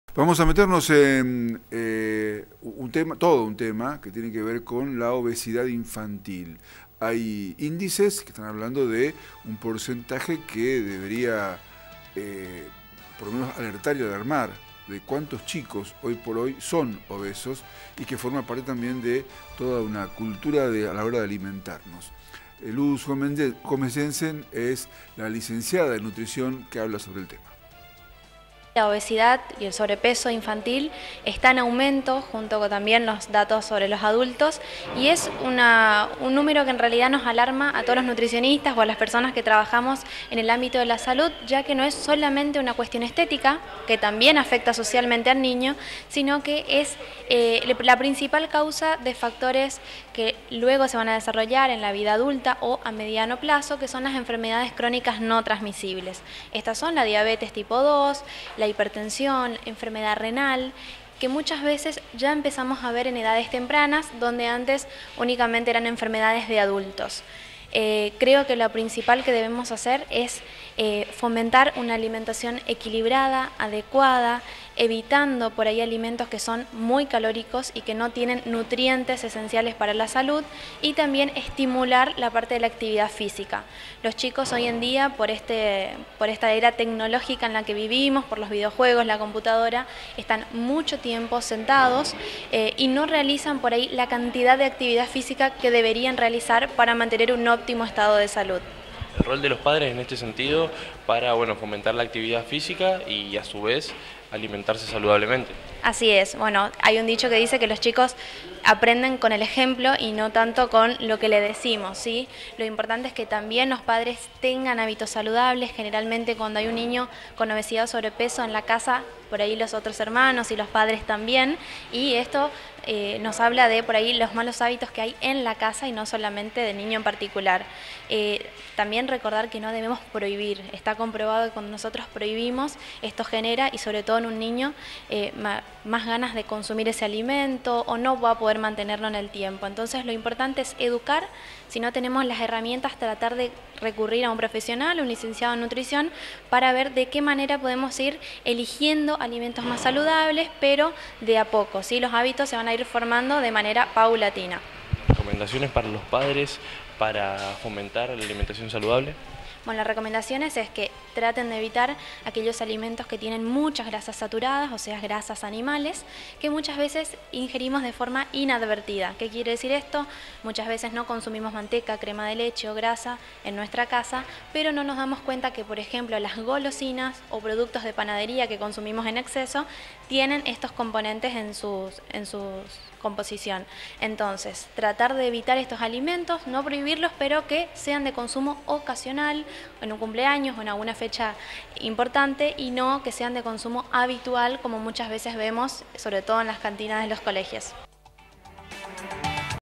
SOMOS CORRIENTES/NOTICIAS
LUZ GOMEZ JENSEN - Lic en nutriciòn
Preocupantes indices de casos de obesidad infantil en laprovncia.Recomendaciones